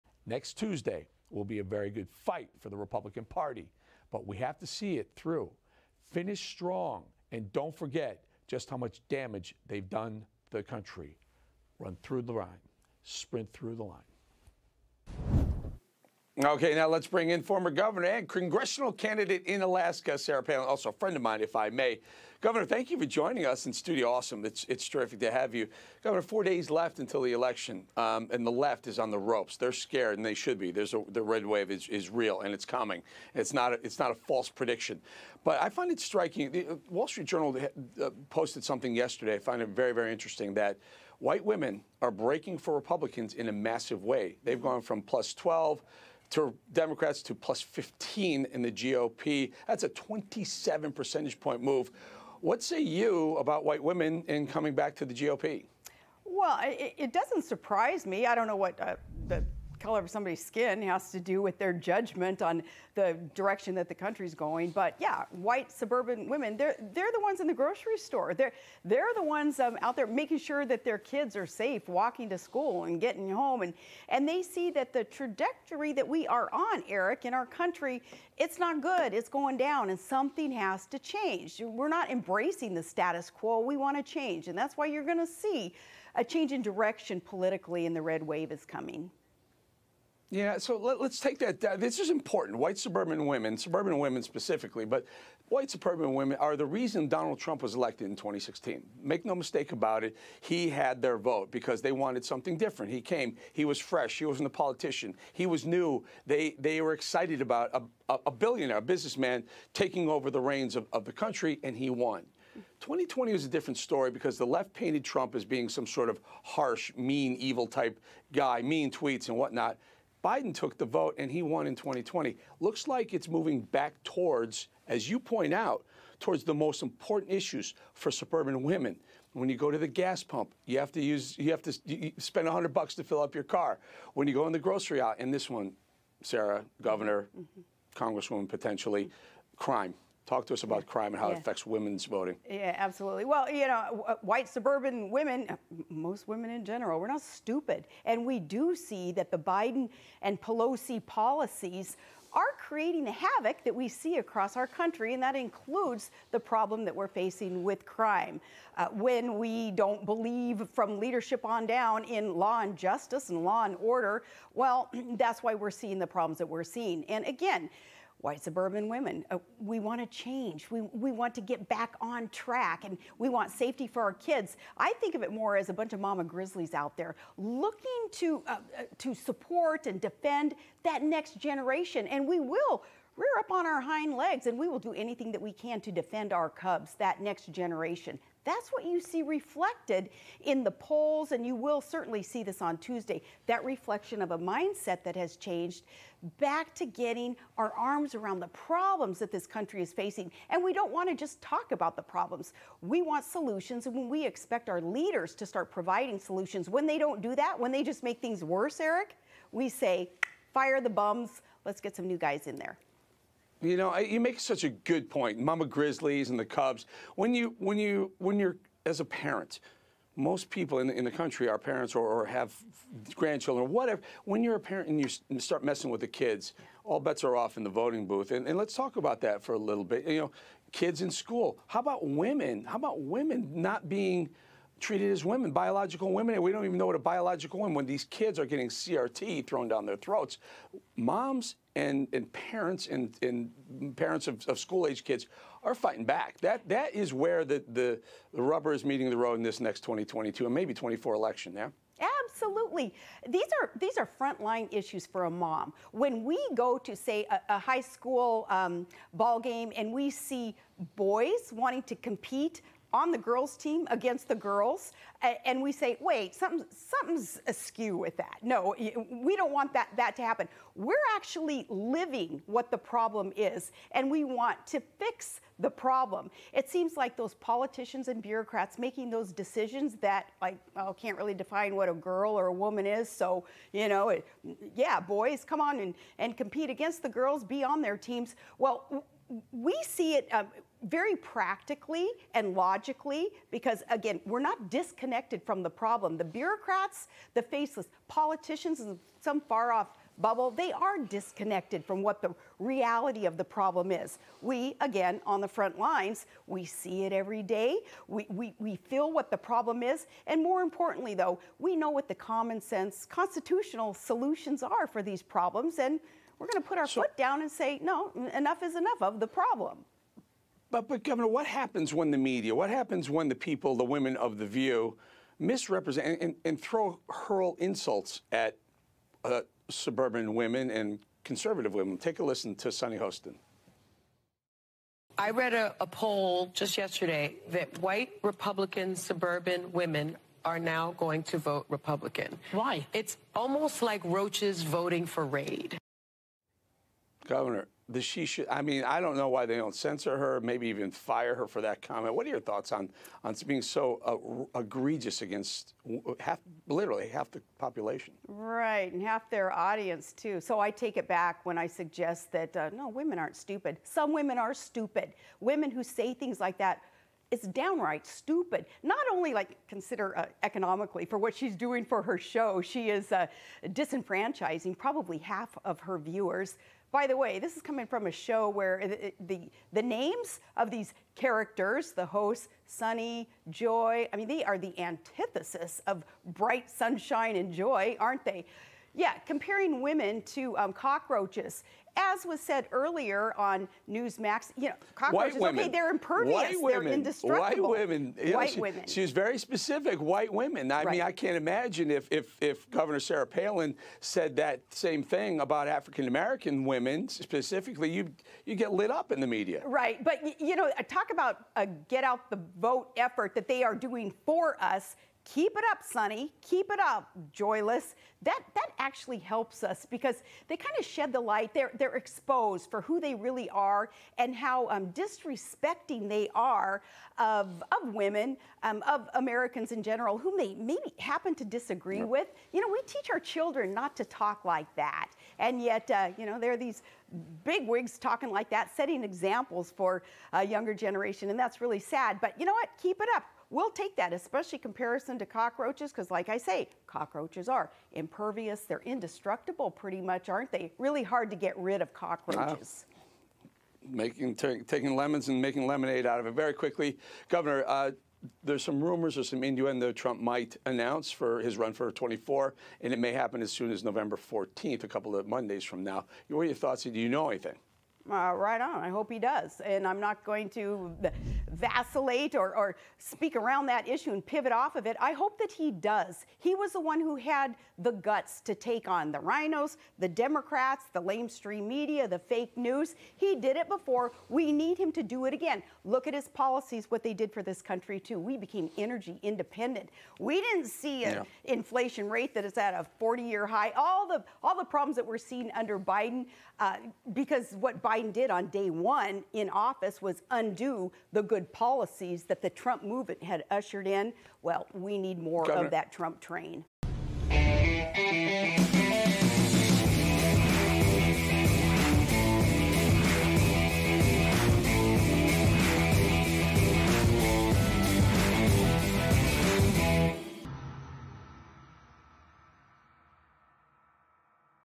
Sarah Palin: Keep it up, 'downright stupid' hosts on The View
Former Governor of Alaska Sarah Palin joins Eric Bolling to discuss the 2022 midterm elections.